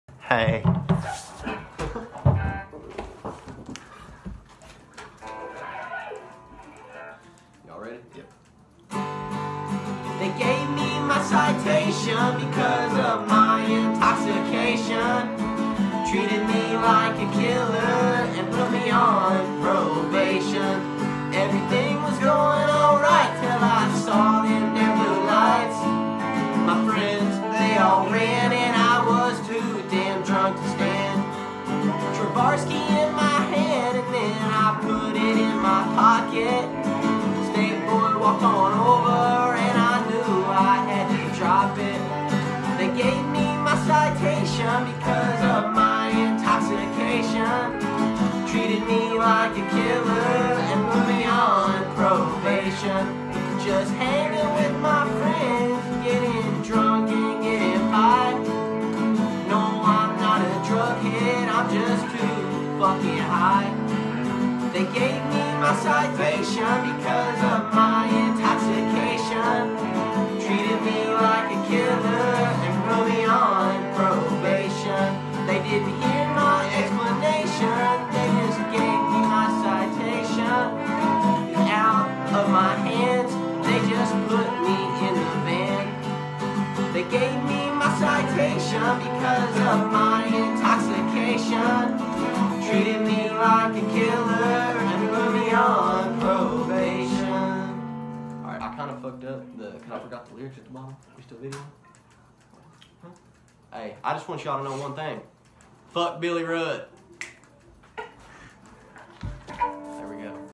Redstring: Dan's Weekend f*k Billy Rudd
Dan got detained at a party had a rough weekend